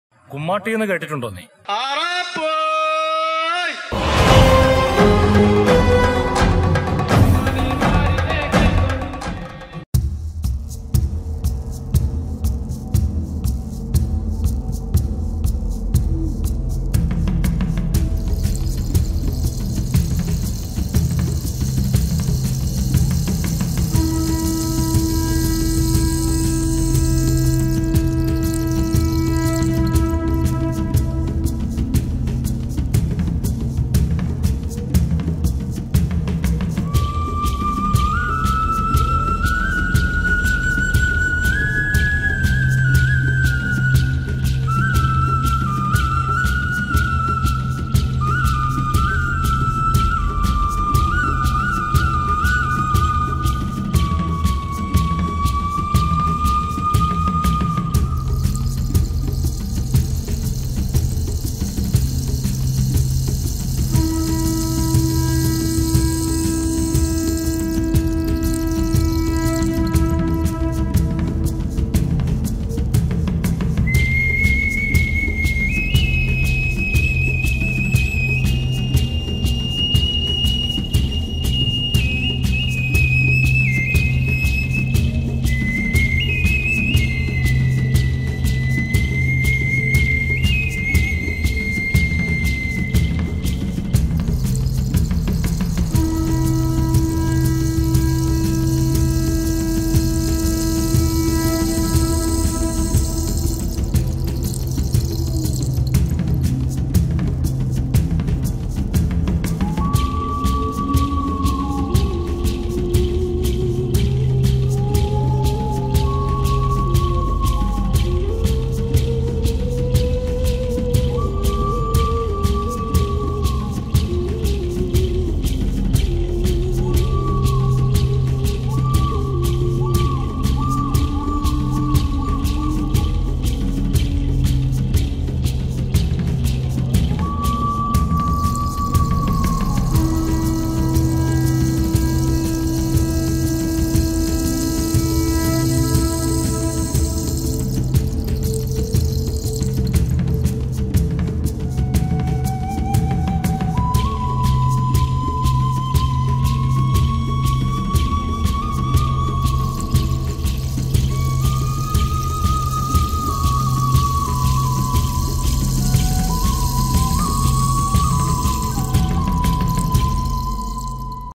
തൃശ്ശൂര് കുമ്മാട്ടി | Kerala folk art | kummatti Thrissur Onam Celebration 2023
Kummaatti is a regional festival celebrated in some parts of Thrissur